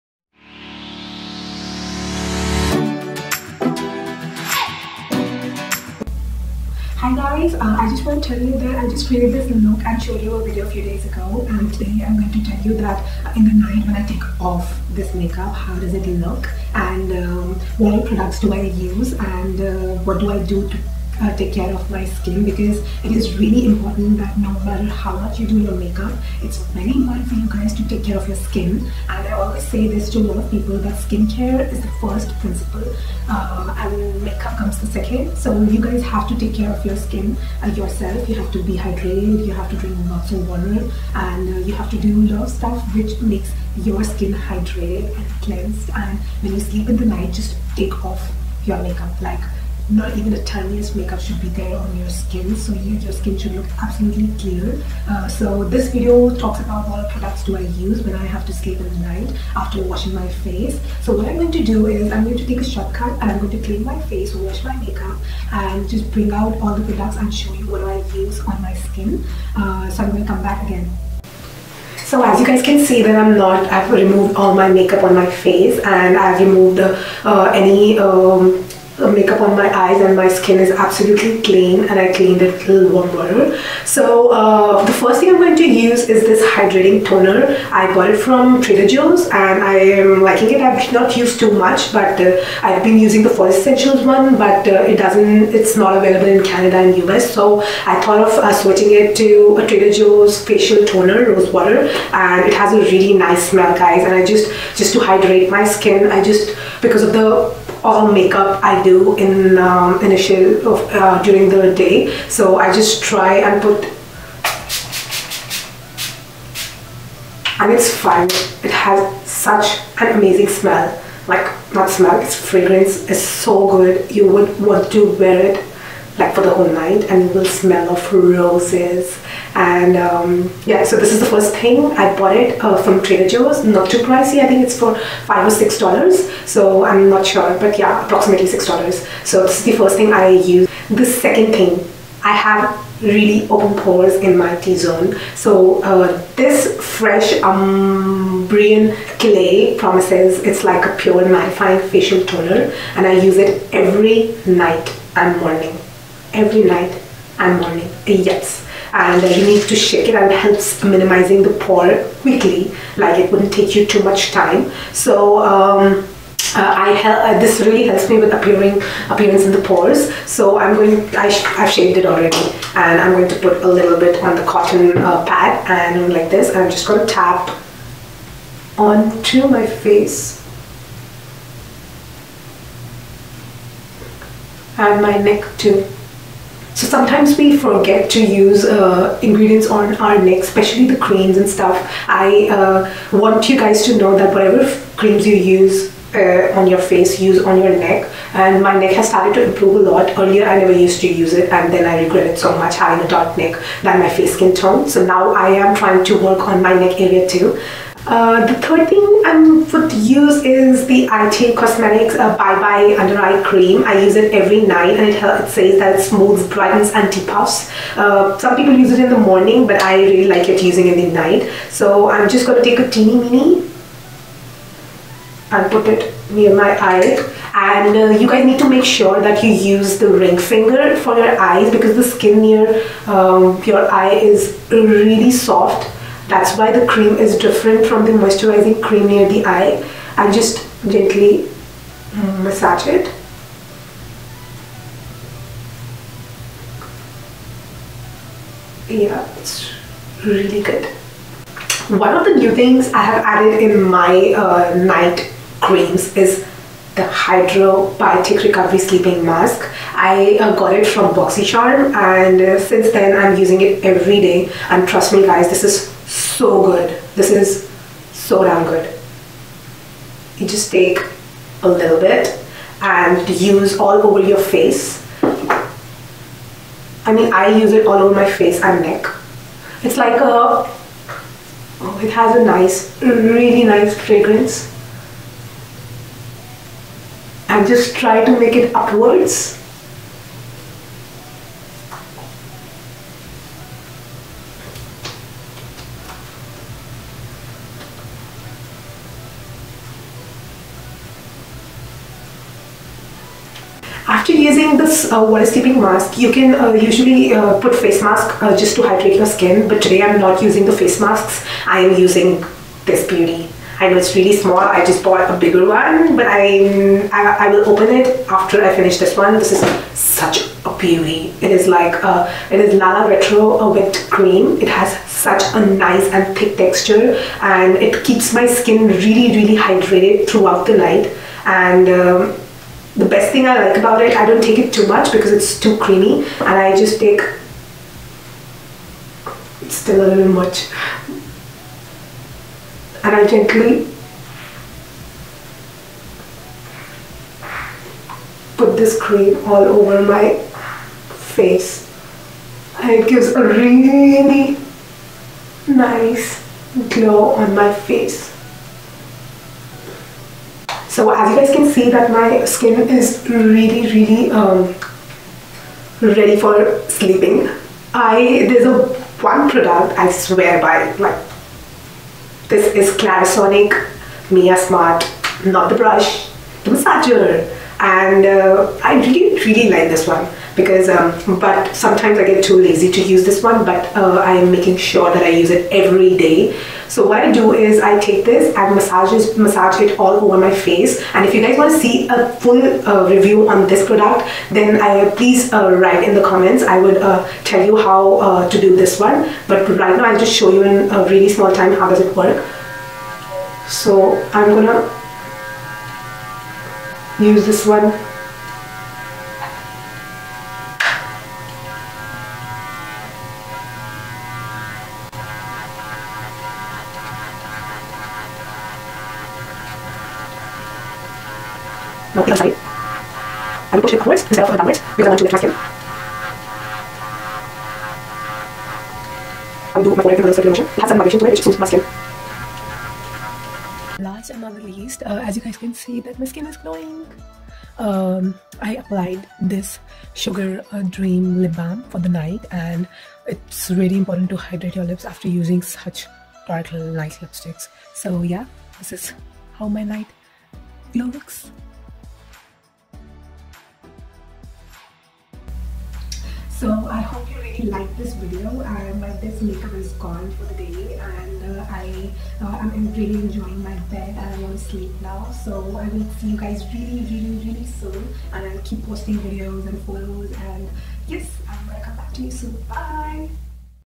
Night Care for face|| Inhousediva
This video talks about is all about the products I have been using during night time before sleep. I have also added some more creams.
The products used are as follows:
1. Facial Toner- Trader Joe's
2. Toner- Fresh umbrian clay
3. hydrating mask- Dr. Brandt sleep recovery mask
4. Cream- Drunk Elephant Lala Retro whipped cream

Love,
 Inhousediva